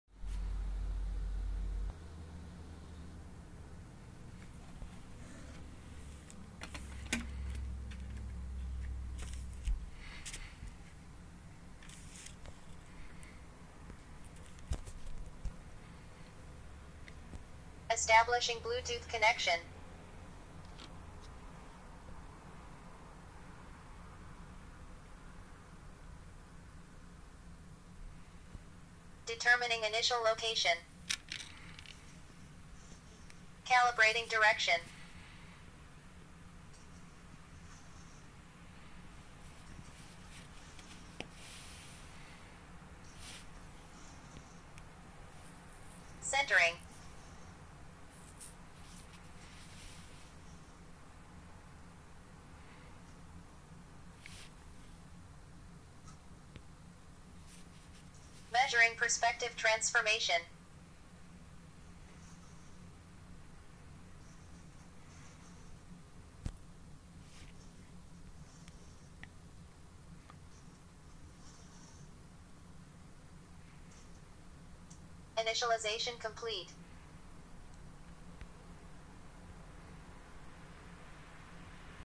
Watchdog for Sphero: initial setup, take two
Using an Android phone as eyes and brain, Sphero orients itself and measures the floor space around it so that the camera viewpoint's perspective transformation can be computed.

Now with 100% more annoying speech synthesis.